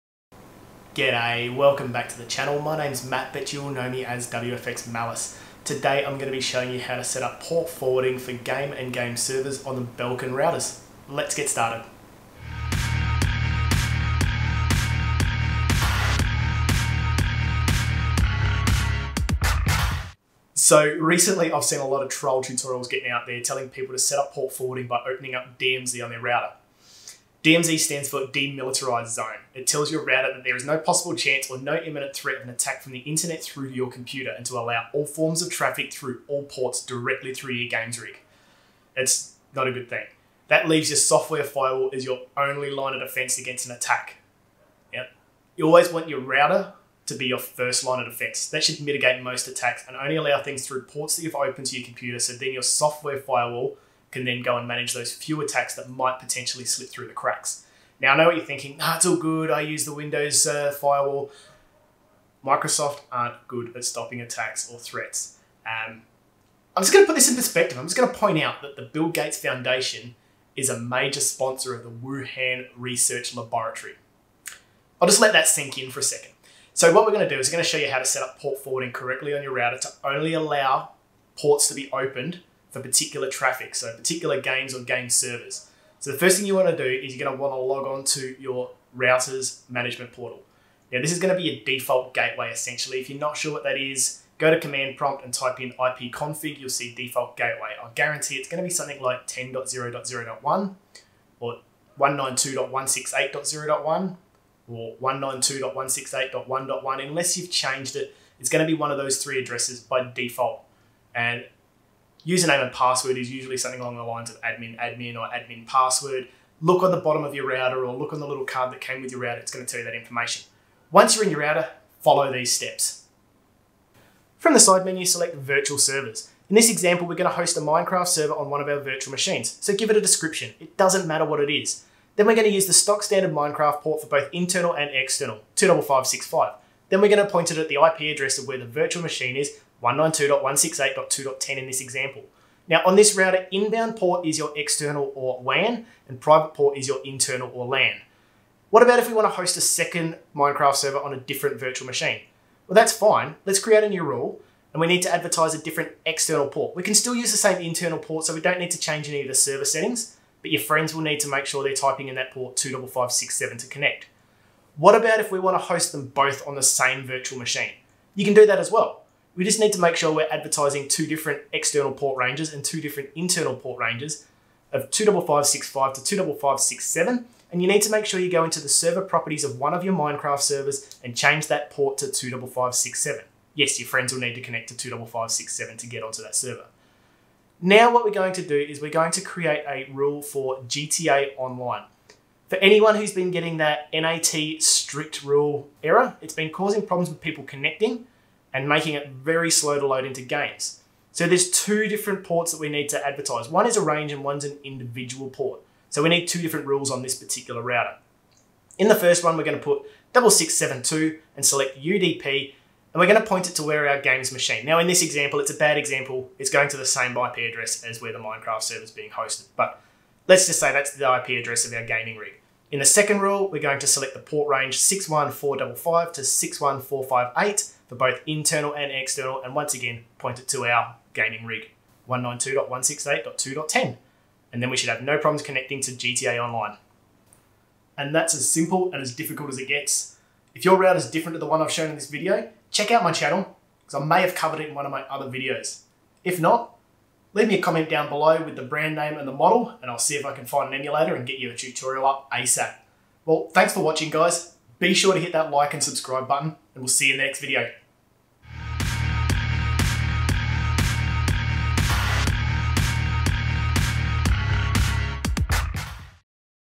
HOW TO: Setup NAT Port Forwarding Rules On Belkin N Series WiFi Routers
If your hosting a game server or trying to connect to a peer-to-peer online game you will more than likely need to set up port forwarding rules on your router. Never fall in to the trap of opening all your ports or enabling DMZ, while this may allow you to connect, it will leave you wide open to probing attacks.

My wish list of accessories for my camera